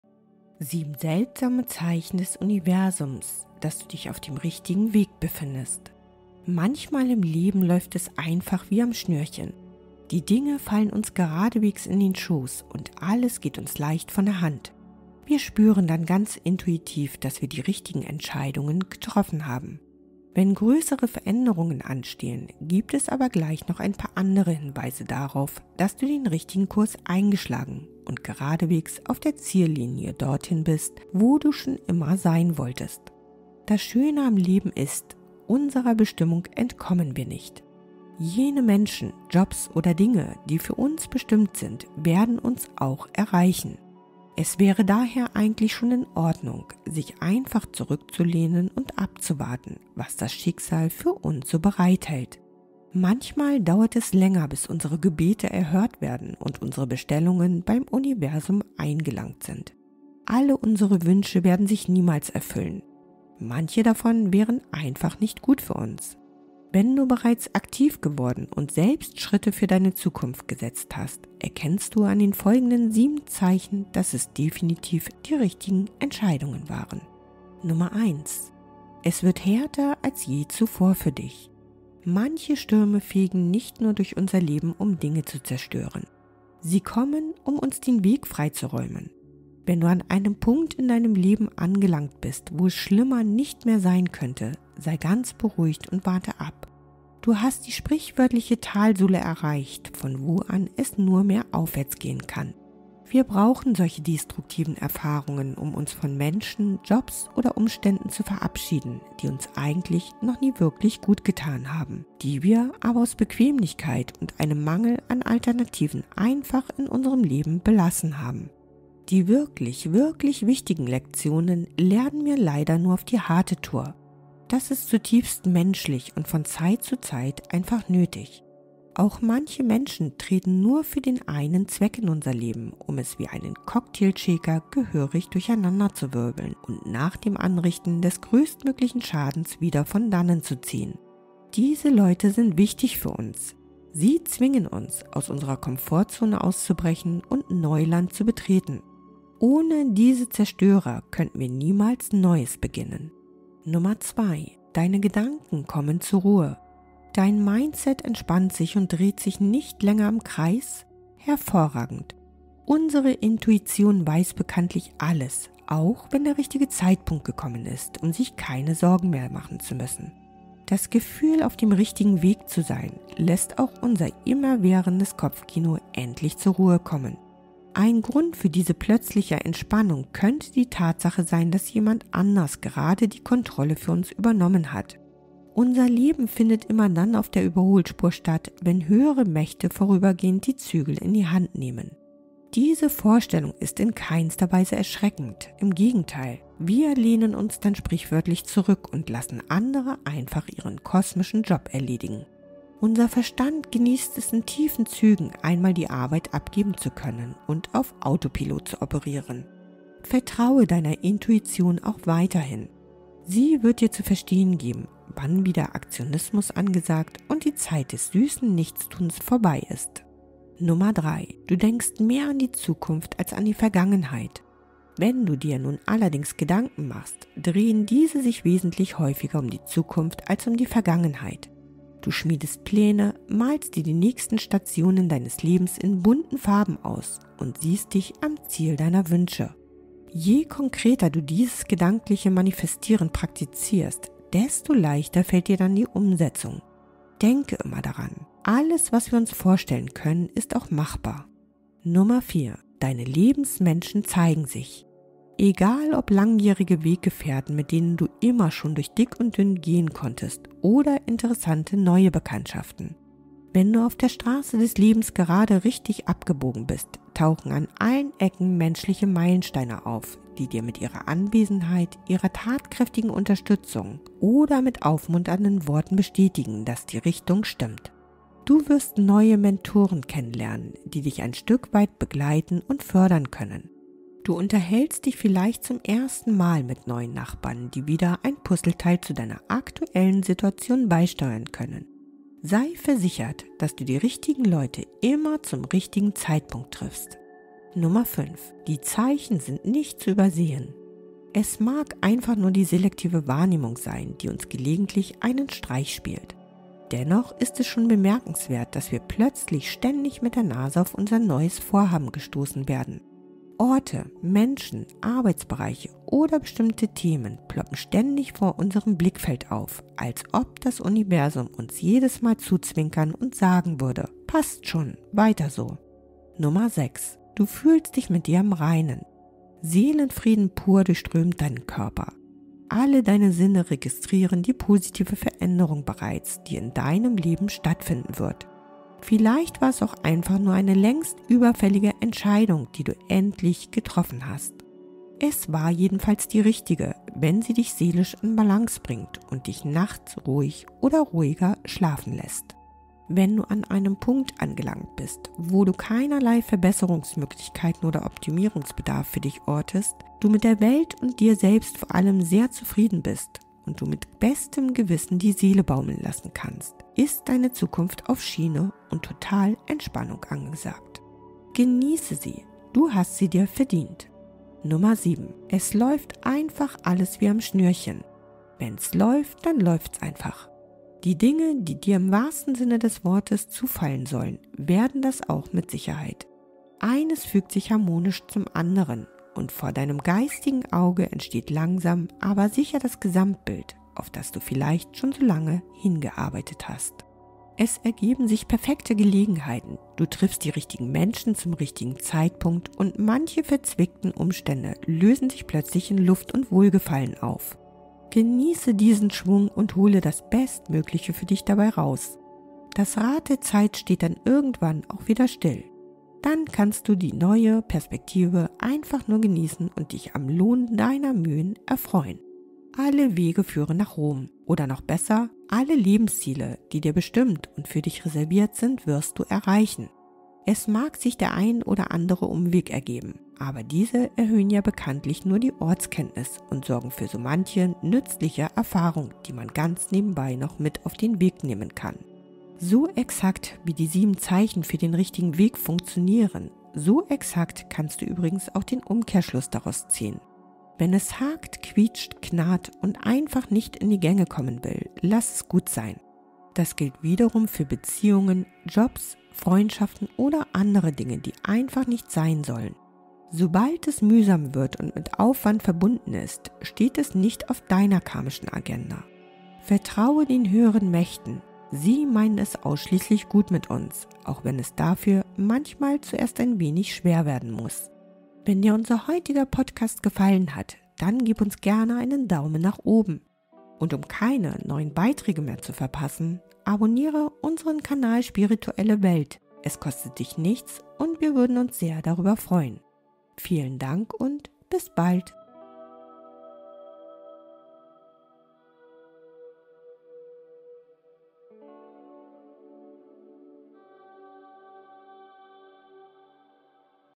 7 seltsame Zeichen des Universums, dass du auf dem richtigen Weg bist!
7 seltsame Zeichen des Universums, dass du auf dem richtigen Weg bist

Manchmal im Leben läuft es einfach wie am Schnürchen. Die Dinge fallen uns geradewegs in den Schoß und alles geht uns leicht von der Hand. Wir spüren dann ganz intuitiv, dass wir die richtigen Entscheidungen getroffen haben. Wenn größere Veränderungen anstehen, gibt es aber gleich noch ein paar andere Hinweise darauf, dass du den richtigen Kurs eingeschlagen und geradewegs auf der Ziellinie dorthin bist, wo du schon immer sein wolltest. Das Schöne am Leben ist: Unserer Bestimmung entkommen wir nicht. Jene Menschen, Jobs oder Dinge, die für uns bestimmt sind, werden uns auch erreichen. Es wäre daher eigentlich schon in Ordnung, sich einfach zurückzulehnen und abzuwarten, was das Schicksal für uns so bereithält. Manchmal dauert es länger, bis unsere Gebete erhört werden und unsere Bestellungen beim Universum eingelangt sind. Alle unsere Wünsche werden sich niemals erfüllen. Manche davon wären einfach nicht gut für uns. Wenn du bereits aktiv geworden und selbst Schritte für deine Zukunft gesetzt hast, erkennst du an den folgenden 7 Zeichen, dass es definitiv die richtigen Entscheidungen waren